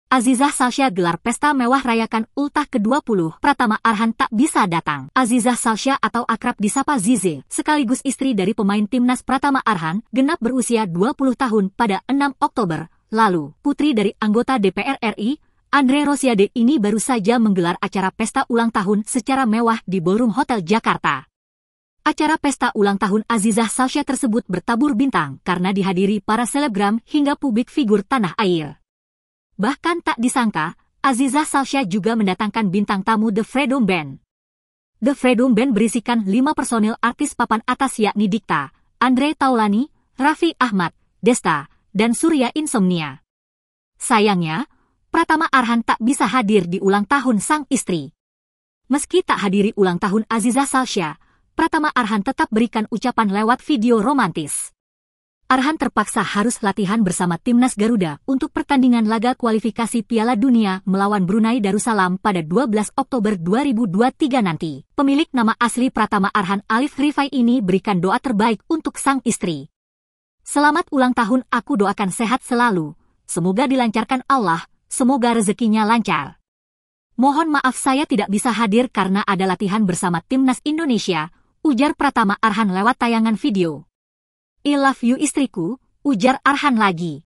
Azizah Salsha Gelar Pesta Mewah Rayakan Ultah ke-20, Pratama Arhan Tak Bisa Datang
TRIBUNSOLO.COM - Azizah Salsha atau akrab disapa Zize, sekaligus istri dari pemain timnas Pratama Arhan, genap berusia 20 tahun pada 6 Oktober, lalu.

Putri dari anggota DPR RI, Andre Rosiade ini baru saja menggelar acara pesta ulang tahun secara mewah di ballroom hotel Jakarta.

Acara pesta ulang tahun Azizah Salsha tersebut bertabur bintang karena dihadiri para selebgram hingga publik figur tanah air.

Bahkan tak disangka, Azizah Salsha juga mendatangkan bintang tamu The Fredoom Band.

The Fredoom Band berisikan lima personil artis papan atas yakni Dikta, Andre Taulany, Raffi Ahmad, Desta, dan Surya Insomnia.

Sayangnya, Pratama Arhan tak bisa hadir di ulang tahun sang istri.

Meski tak hadiri ulang tahun Azizah Salsha, Pratama Arhan tetap berikan ucapan lewat video romantis.

Arhan terpaksa harus latihan bersama Timnas Garuda untuk pertandingan laga kualifikasi Piala Dunia melawan Brunei Darussalam pada 12 Oktober 2023 nanti.

Pemilik nama asli Pratama Arhan Alif Rifai ini berikan doa terbaik untuk sang istri.

"Selamat ulang tahun aku doakan sehat selalu, semoga dilancarkan Allah, semoga rezekinya lancar.

Mohon maaf saya tidak bisa hadir karena ada latihan bersama Timnas Indonesia," ujar Pratama Arhan lewat tayangan video.

"I Love you istriku," ujar Arhan lagi.

Artikel ini telah tayang di Tribunnews.com dengan judul Meski Tak Hadiri Ultah Azizah Salsha, Pratama Arhan Beri Ucapan Video Romantis:I Love You Istriku,
Penulis: Rinanda DwiYuliawati